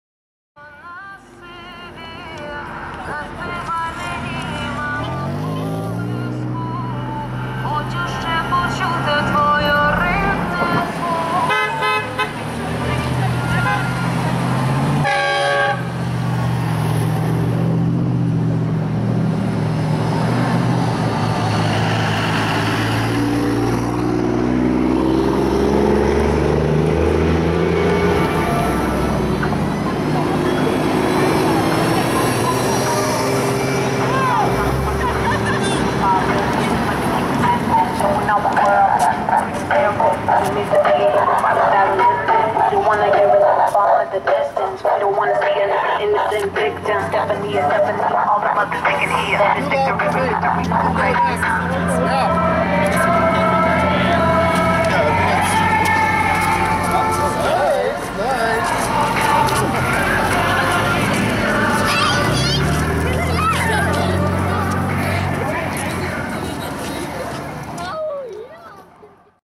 walk with music
Participants in the May 19 Walk for Ukraine cross the Portage Lift Bridge from Houghton to Hancock displaying signs and flags and some wearing traditional dress in honor of a Ukrainian holiday. (Video by Keweenaw Now)